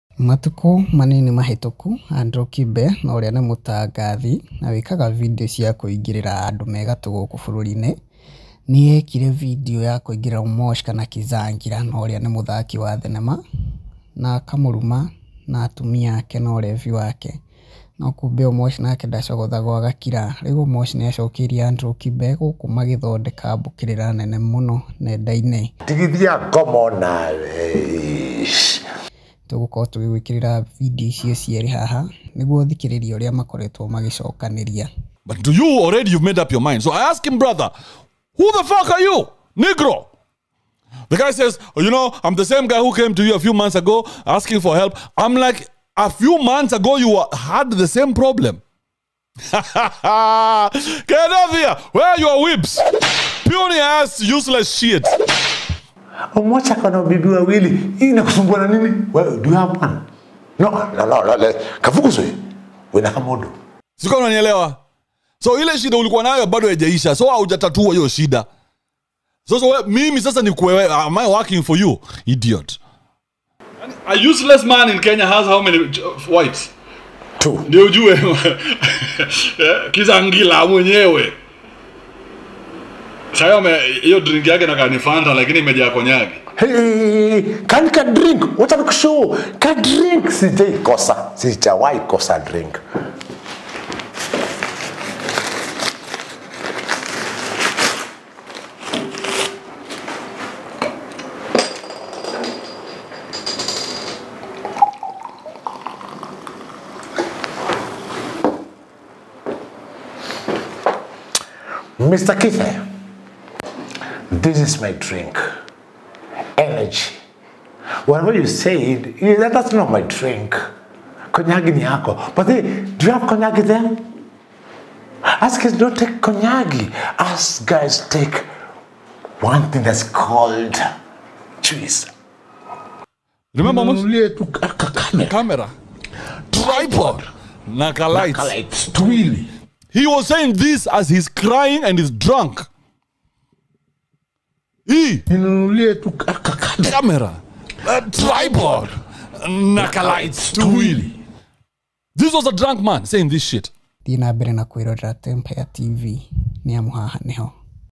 "NDIGITHIA NGOMA." OMOSH TELLS OFF ANDREW KIBE
@kibeandy @omoshkizangila Mr Lambistic